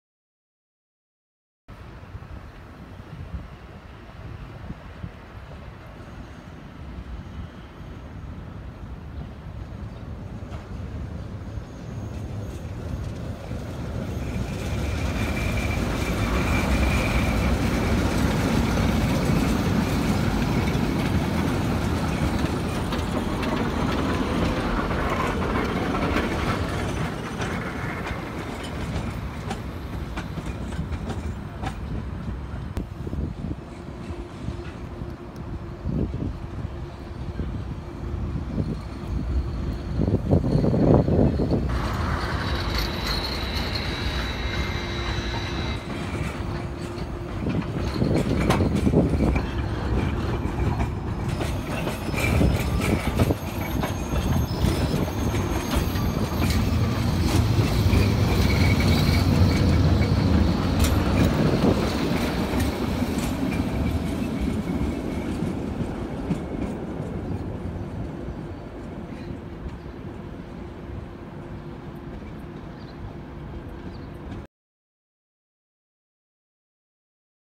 Трамвай ЛВС-97К (71-147К) Борт №1028
Трамвайный Вагон Приписан в
Трамвайный/Парк №1
Следуя по Маршруту №62
На Конечной/Остановки
"Станция Метрополитена "Купчино""
До Конечной/Остановки
"Малая Балканская Улица"
Отправляется от Конечной/Остановки
"Станция Метрополитена "Купчино""
Фрунзенский Район,
Санкт-Петербург,Россия.
18 Июня 2019 Года.(11:13-11:16).